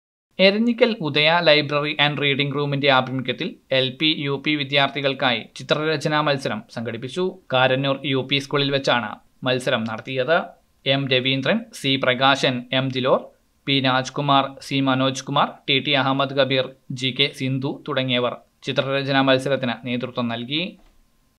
എൽ പി യുപി വിദ്യാർത്ഥികൾക്കായി ചിത്രരചന മത്സരം
എരഞ്ഞിക്കൽ ഉദയ ലൈബ്രറി ആൻഡ് റീഡിങ് റൂമിന്റെ ആഭിമുഖ്യത്തിൽ എൽ പി  യുപി വിദ്യാർത്ഥികൾക്കായി ചിത്രരചന മത്സരം സംഘടിപ്പിച്ചു.കാരന്നൂർ യുപി സ്കൂളിൽ വെച്ചാണ് മത്സരം നടത്തിയത്